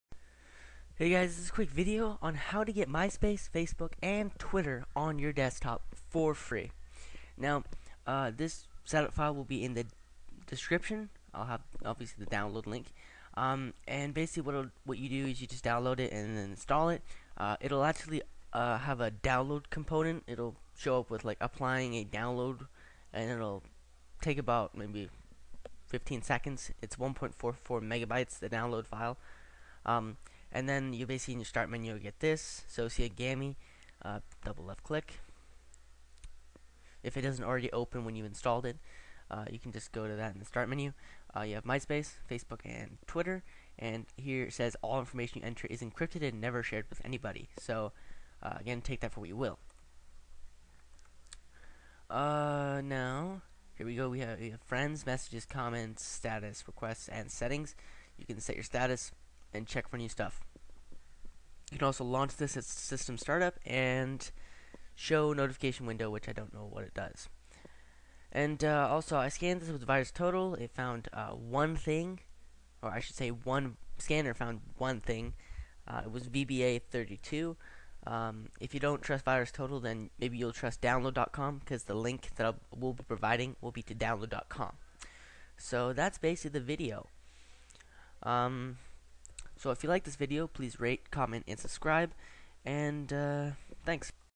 Login in to Myspace, Twitter, and Facebook at the Same Time
This video shows a program which will give you access to Facebook, Myspace, and Twitter on your desktop. The program is called Sociagami
Sociagami